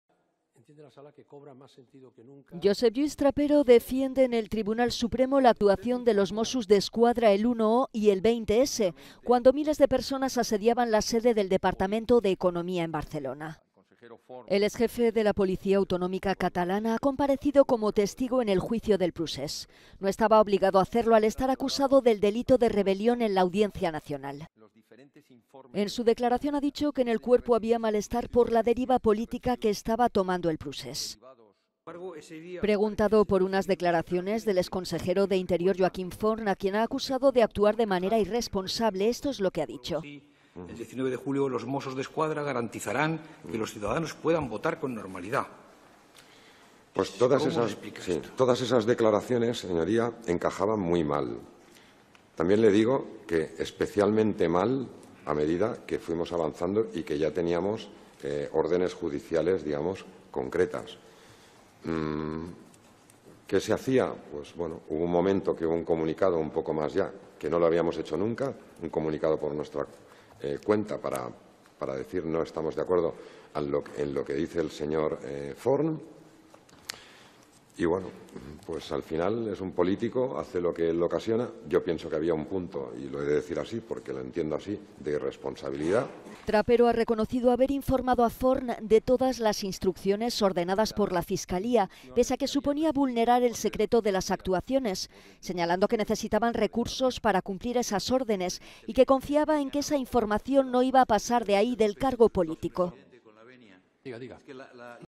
Trapero acusa a Forn de actuar con "irresponsabilidad"
Josep Lluís Trapero, exjefe de los Mosso d'Esquadra, declara a las 11:00 hora local, ante el Tribunal Supremo como testigo en el juicio a los independentistas catalanes.…